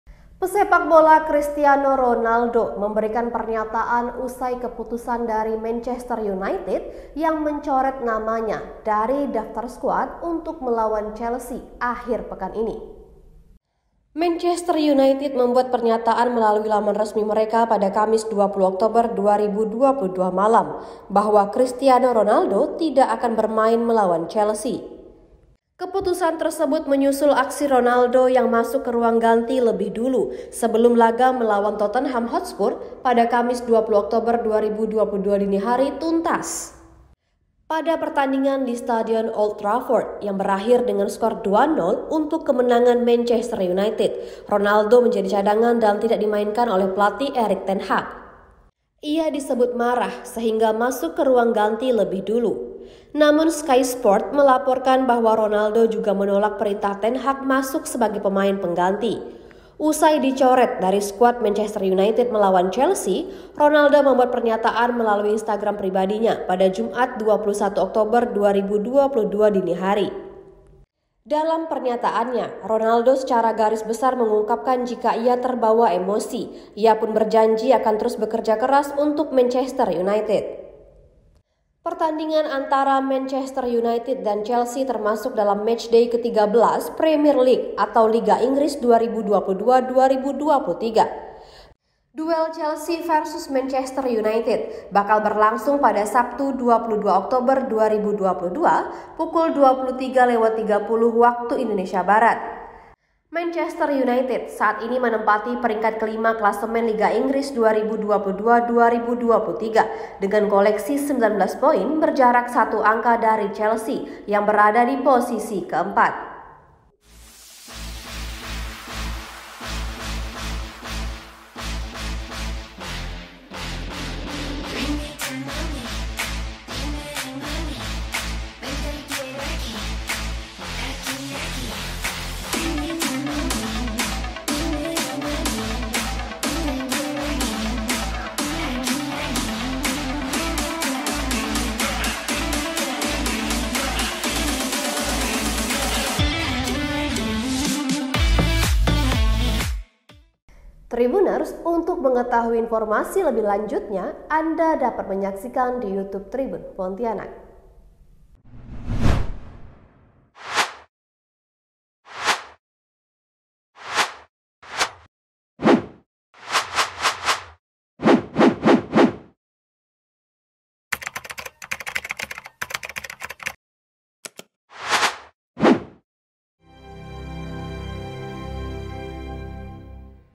🔴 Pernyataan Cristiano Ronaldo Usai Dicoret dari Skuad Manchester United Lawan Chelsea
Cristiano Ronaldo memberikan pernyataan usai keputusan Manchester United mencoretnya dari daftar skuad melawan Chelsea akhir pekan ini.  Manchester United membuat pernyataan melalui laman resmi mereka pada Kamis 20 Oktober 2022 malam WIB bahwa Cristiano Ronaldo tidak akan bermain melawan Chelsea.  "Cristiano Ronaldo tidak akan menjadi bagian dari skuad Manchester United untuk pertandingan Premier League Sabtu ini melawan Chelsea. Anggota skuad lainnya sepenuhnya fokus pada persiapan untuk pertandingan itu," demikian pernyataan klub.  Keputusan Man United itu menyusul aksi Ronaldo yang masuk ke ruang ganti lebih dulu sebelum laga melawan Tottenham Hotspur, Kamis 20 Oktober 2022 dini hari WIB tuntas.

Pada pertandingan di Stadion Old Trafford yang berakhir 2-0 untuk kemenangan Man United, Ronaldo menjadi cadangan dan tidak dimainkan pelatih Erik ten Hag.  Ia disebut marah sehingga masuk ke ruang ganti lebih dulu. Namun, Sky Sports melaporkan bahwa Ronaldo juga menolak perintah Ten Hag masuk sebagai pemain pengganti.  Usai dicoret dari skuad Man United melawan Chelsea, Ronaldo membuat pernyataan melalui Instagram pribadinya, Jumat 21 Oktober 2022 dini hari WIB.  "Seperti yang selalu saya lakukan sepanjang karier saya. Saya menghormati rekan-rekan saya, lawan, dan pelatih saya. Itu tidak pernah berubah," kata Ronaldo.

"Saya mempertahankan sikap ini selama 20 tahun terakhir karier saya di sepak bola elite. Rasa hormat selalu menjadi peran penting dalam pengambilan keputusan saya," ucapnya.

Dalam pernyataannya, Ronaldo secara garis besar mengungkapkan bahwa ia terbawa emosi. Ia pun berjanji akan terus bekerja keras untuk Manchester United.

"Saya mengawali karier pada usia sangat muda dan penting bagi saya mencontoh pemain yang lebih berpengalaman. Oleh karena itu, saya selalu mencoba melakukan hal yang sama kepada pemain-pemain muda," kata Ronaldo.  "Sayangnya, terkadang emosi menguasai kami. Saat ini, saya merasa harus terus bekerja keras di Carrington, mendukung tim saya, dan siap untuk segalanya dalam pertandingan apa pun," ucap Ronaldo.  "Menyerah pada tekanan tidak pernah menjadi pilihan. Ini Manchester United. Kita akan bersama lagi secepatnya," ucapnya.

Pertandingan antara Man United dan Chelsea termasuk dalam matchday ke-13 Premier League atau Liga Inggris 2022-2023.  Duel Chelsea vs Man United bakal berlangsung di Stadion Stamford Bridge, Sabtu 22 Oktober 2022 pukul 23.30 WIB.  Man United saat ini menempati peringkat kelima klasemen Liga Inggris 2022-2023 dengan koleksi 19 poin, berjarak satu angka dari Chelsea yang berada di posisi keempat.